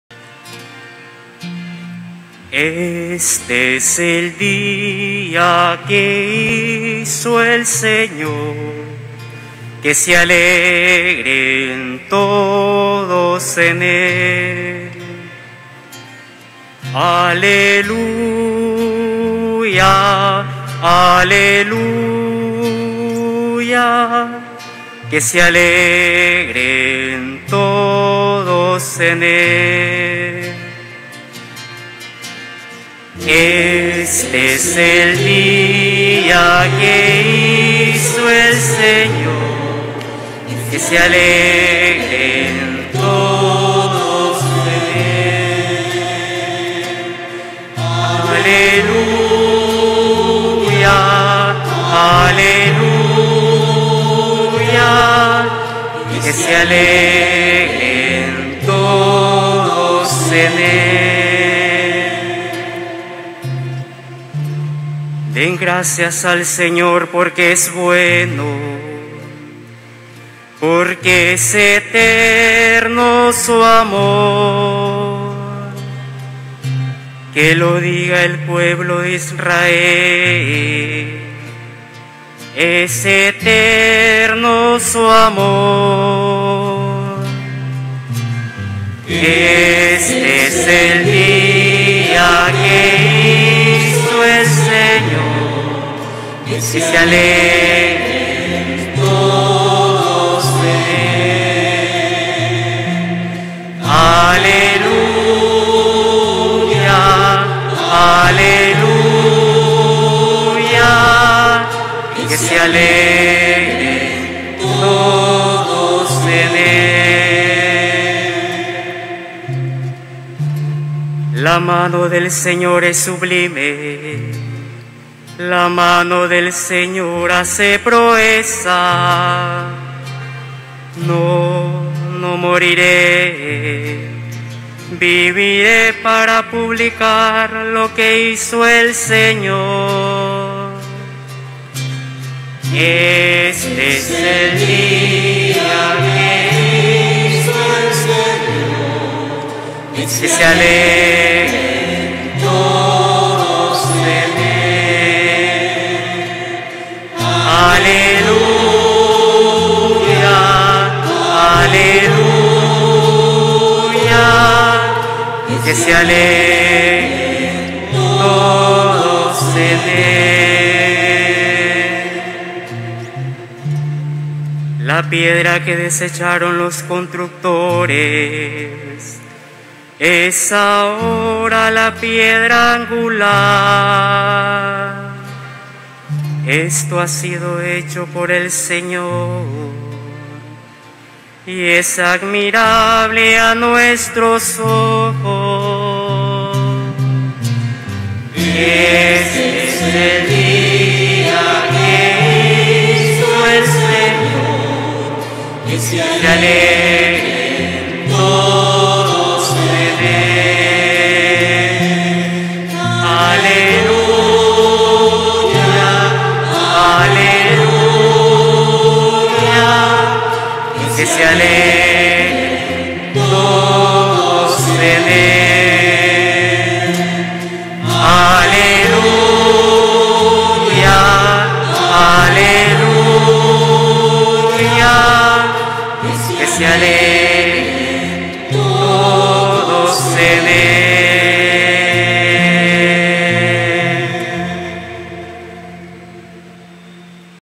Salmo Domingo Misericordia
Lectura y entonación del Salmo en celebración del Domingo de la Misericordia en Catedral de Granada.